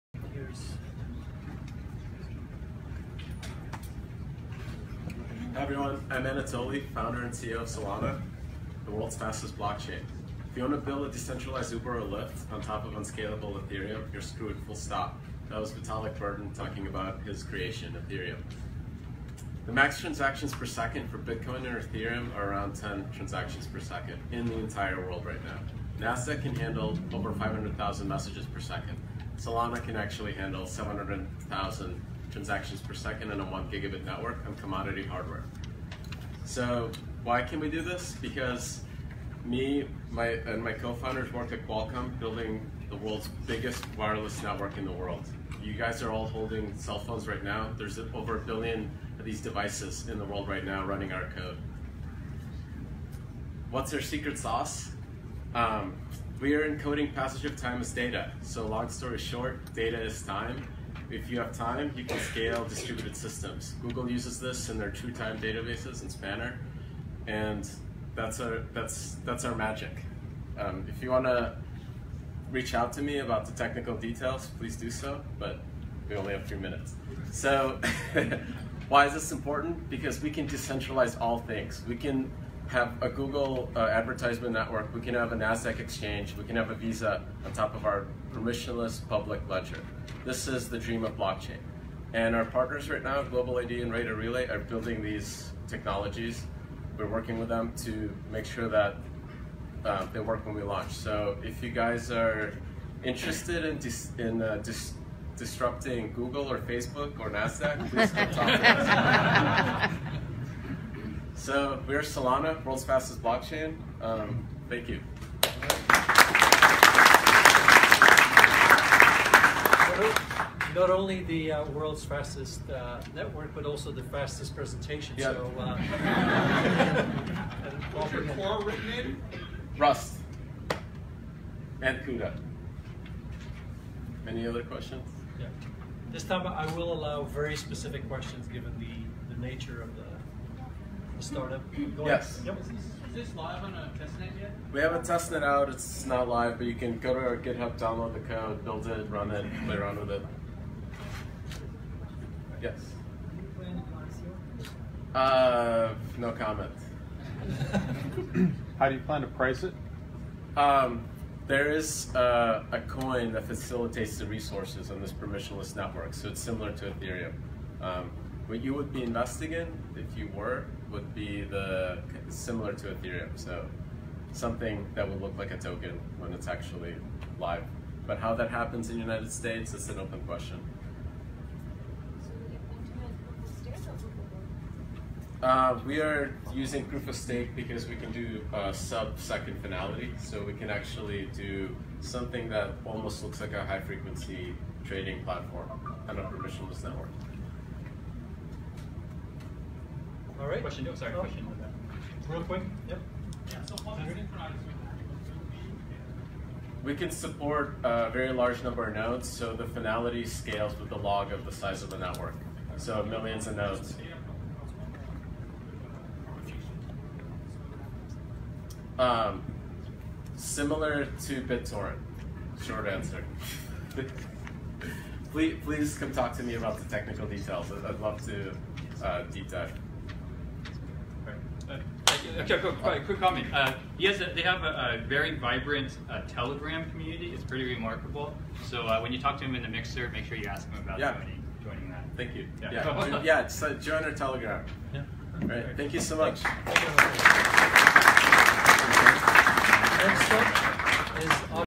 Solana: The World's Fastest Blockchain (elevator pitch at Xoogler Demo Day)
Find out more at solana.io!
In April of 2018 we attended a pitch session at Google Headquarters in SF. Alan, our BD leader, is a longtime former Googler and brought us in for a brief 3 minute pitch. We think this is a nice overview if you want a quick summary of Solana.